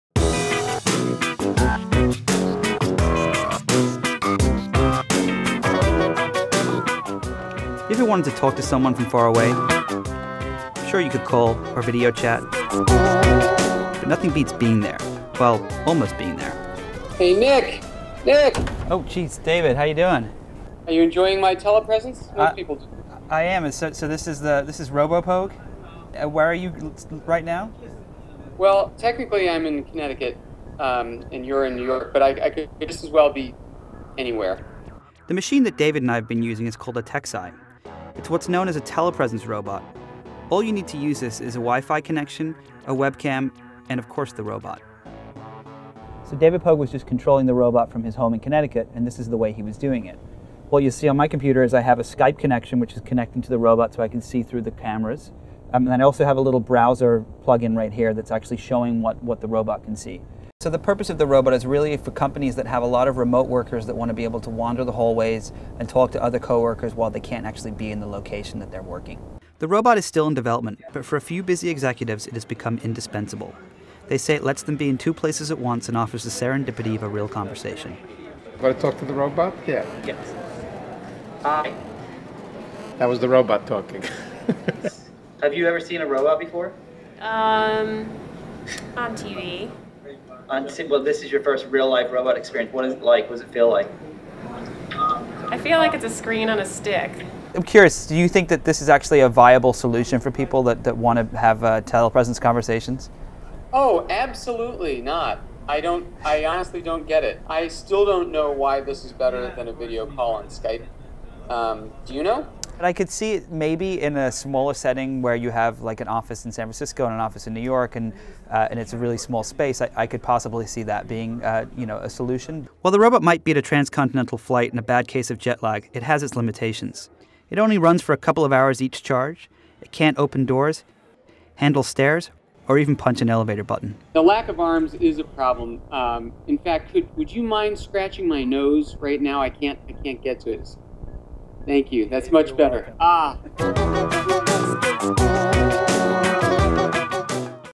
Science: Can't Attend? Send Your Robot Instead
Nick Bilton and David Pogue try out a new telepresence robot that allows users to remotely drive a video chat screen down hallways and to meetings.